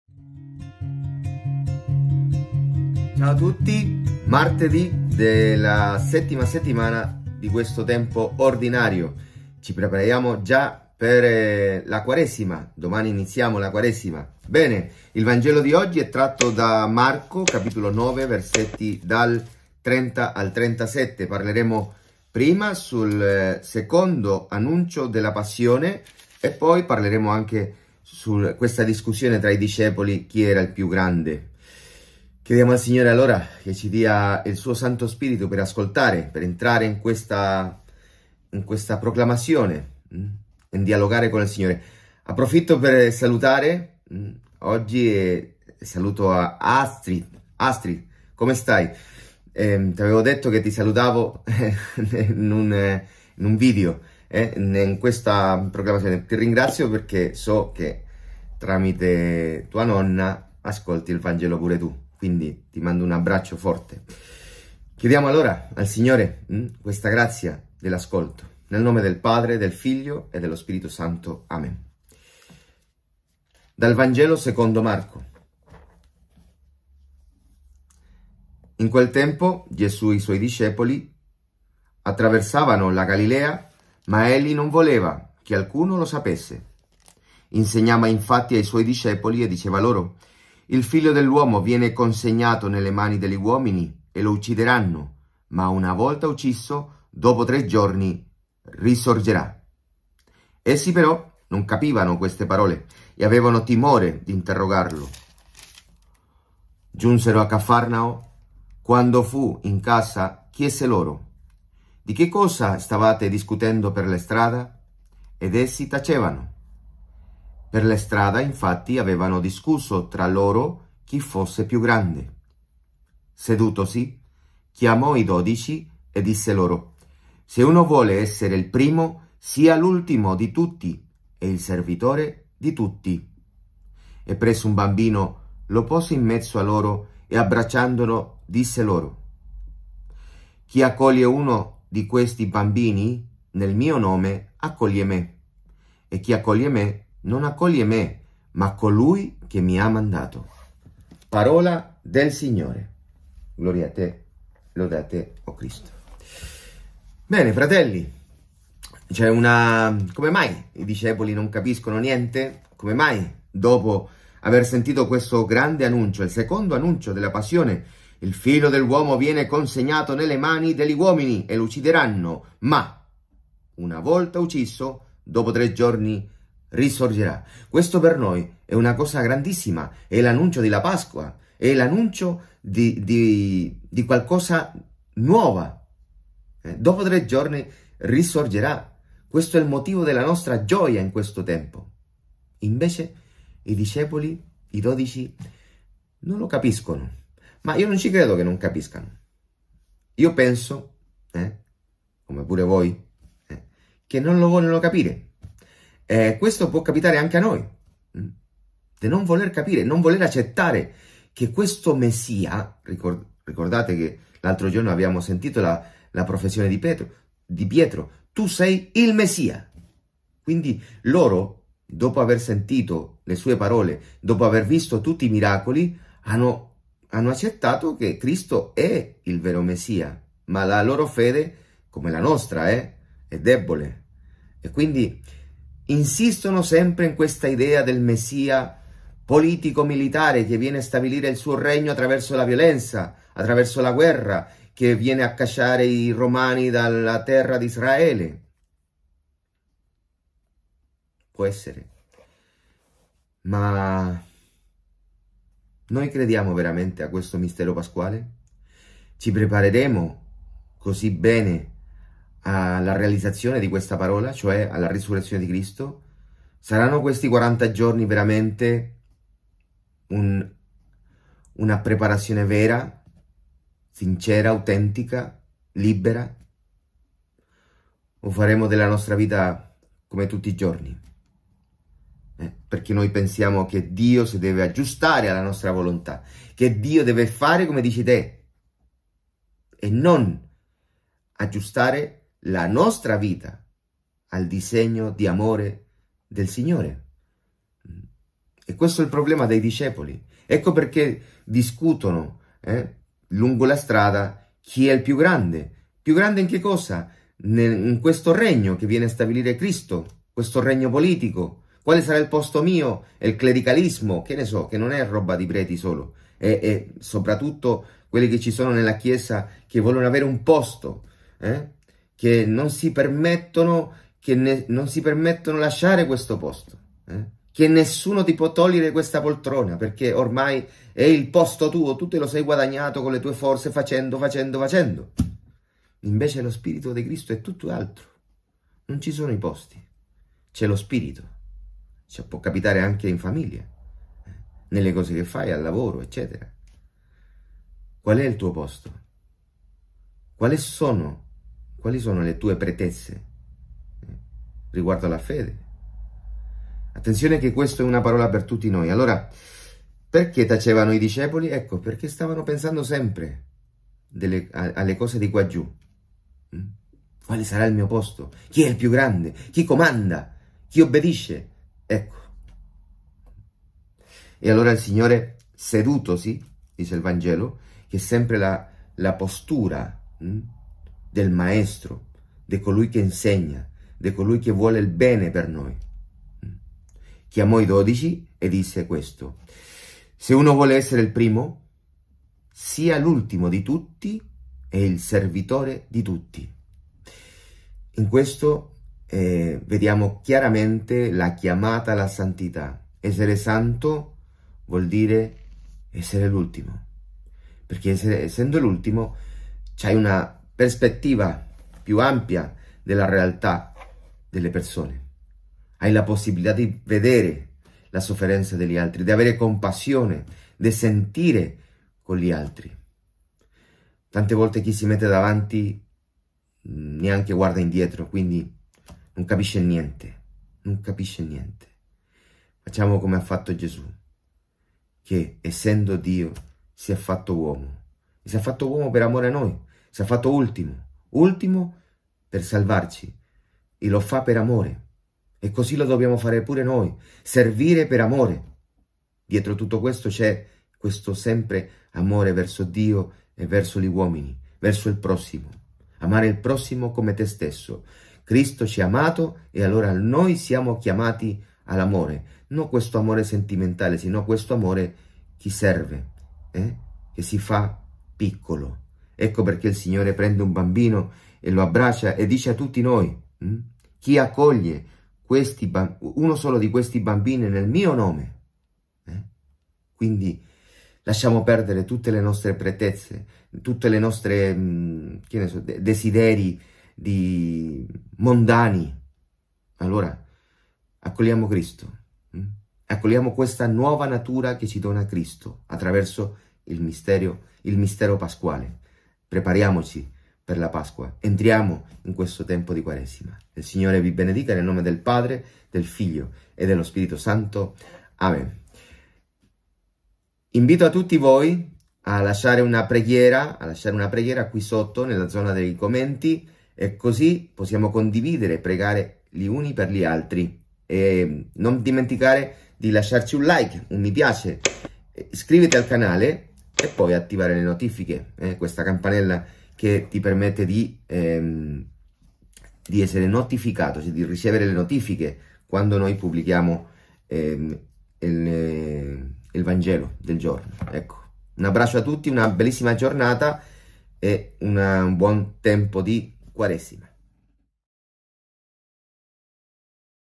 Vangelo del giorno martedì 21 febbraio. Commento di don Felipe Brito | Liturgia della Parola
vangelodelgiorno secondo Marco 9, 30-37
CHI È IL PIÙ GRANDE?
Martedì VII Settimana del Tempo Ordinario